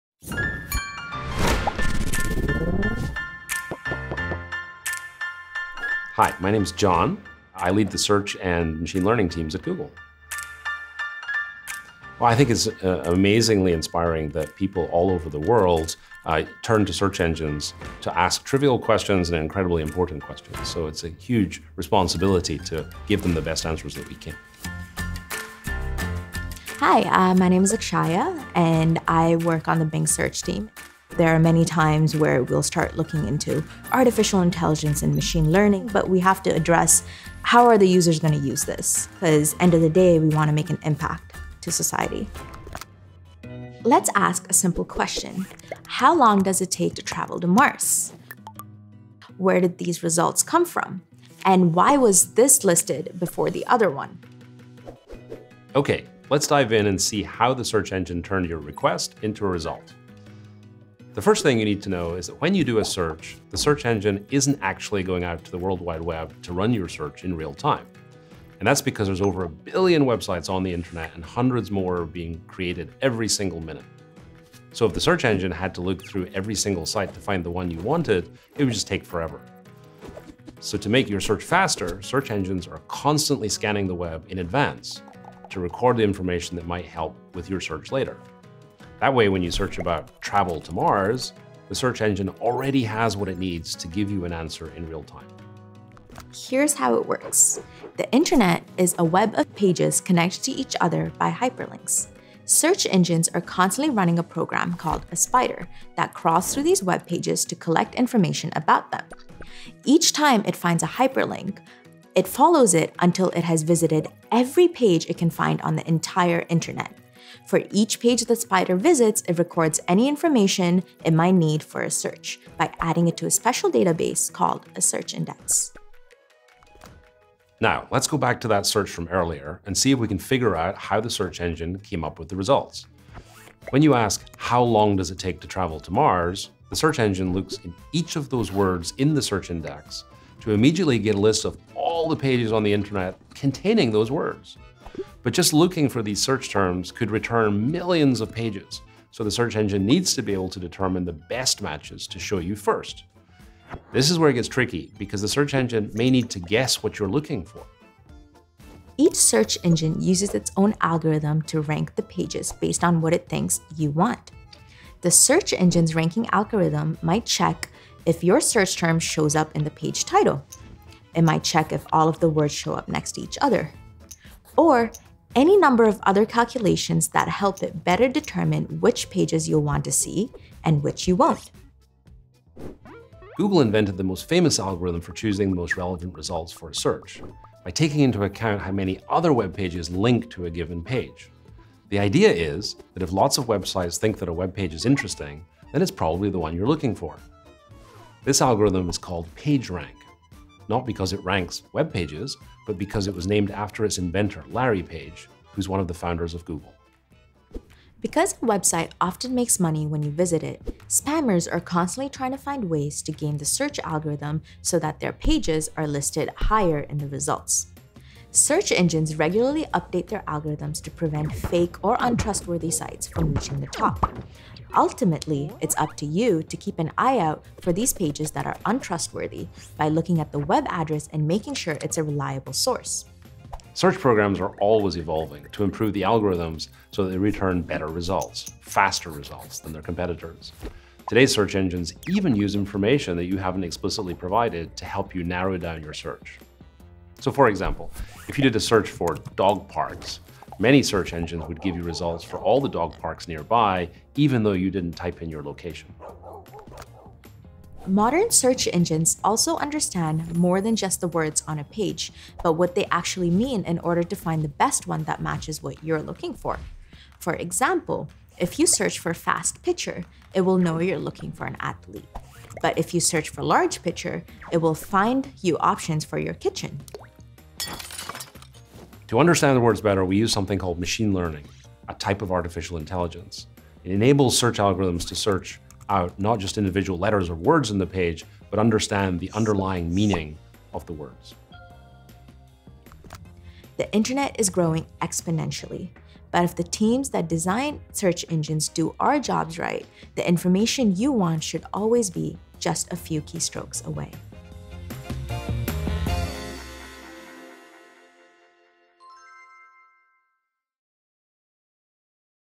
The Internet: How Search Works
Join John, Google's Chief of Search and AI, and Akshaya, from Microsoft Bing, to find out how search really works. They cover everything from how special programs called "spiders" scan the Internet before you even type in your search terms to what determines which search results show up first. Find out how search algorithms bust spammers, manage location services and even use machine learning to make search better every year.